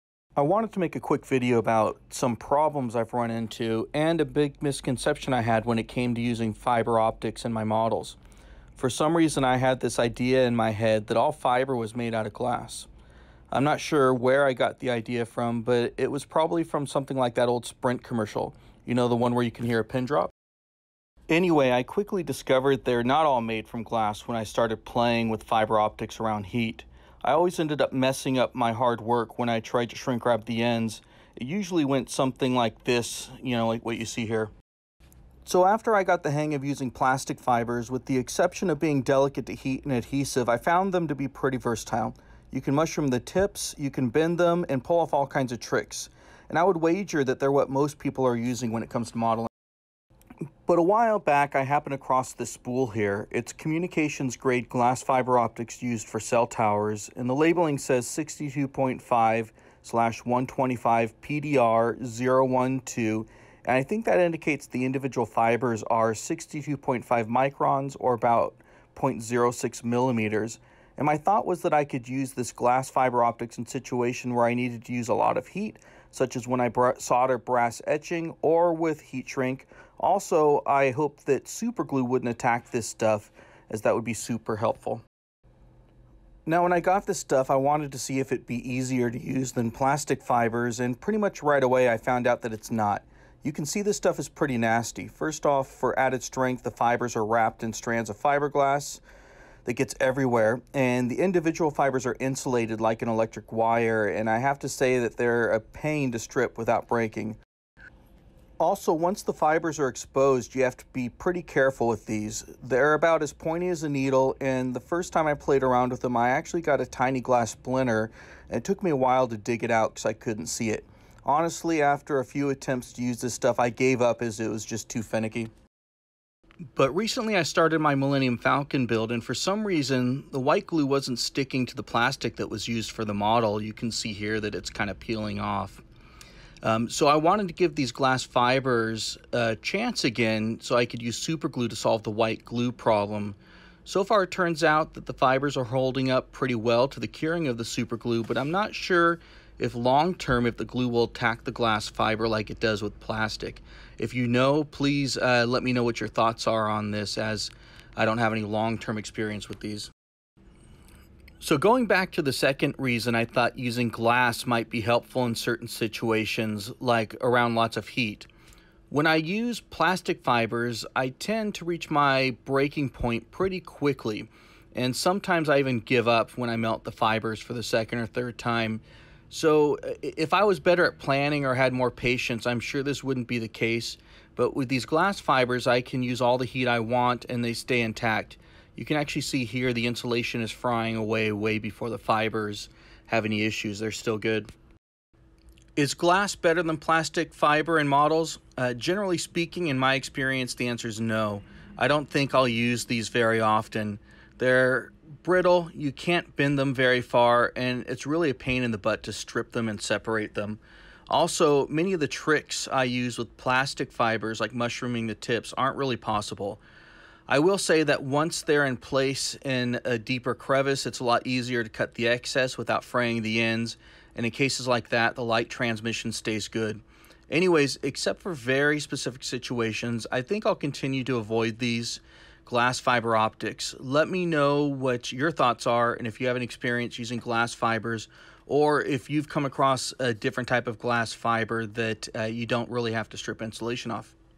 Glass Fiber Optics for Scale Models
Are glass fiber optics easier or better to use in our scale models?